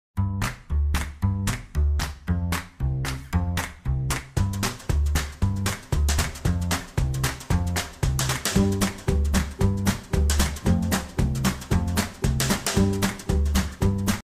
Clip on strainer, snap strainer, cooking strainer for pasta, pot and pan
Clip on strainer

UNIVERSAL DESIGN - The specially designed clips will fit nearly all round pots, pans and bowls big and small (yes, even the ones with a lip).
SPACE SAVING - Small, compact strainer that is easy to use and store in a quarter the size of the traditional colander. Great when working with limited counter space.